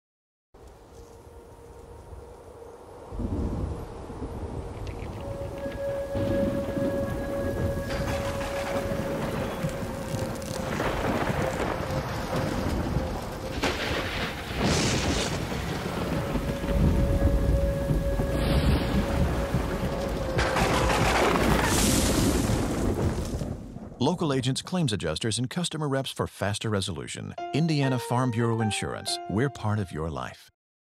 Insurance "HOME LIGHTNING STORM" TV spot
Insurance spot shot on a RED ONE camera. We used several LIGHTNING STRIKES units to create the lightning flashes. Rain trees & high-velocity wind machines help create the mood of the impending storm. RED was mounted on a CAM-MATE crane and flown thru the rain! Since no one yet makes any type of RED ONE rain covers, we put our faith in the trusty Hefty garbage bags! Thank goodness it was an insurance spot! BTW, RED works GREAT with the Lightning Strikes! Zero rolling shutter effects!!